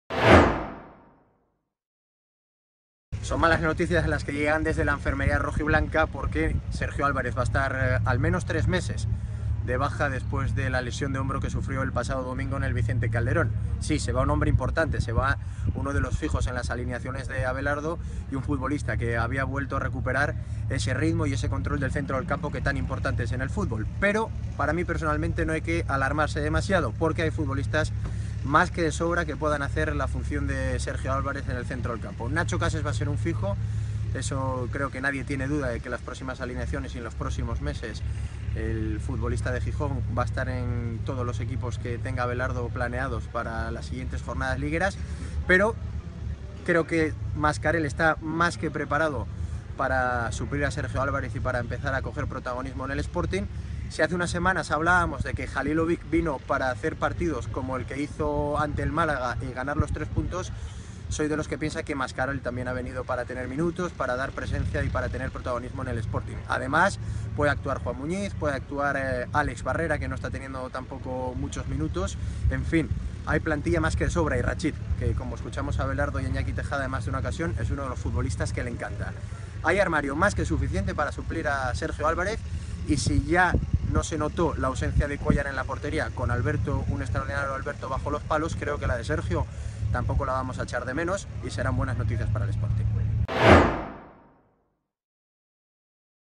Andrés Maese: "La baja de Sergio estará bien cubierta"
Entrada en el videoblog de Andrés Maese sobre la grave lesión del esportinguista Sergio Álvarez.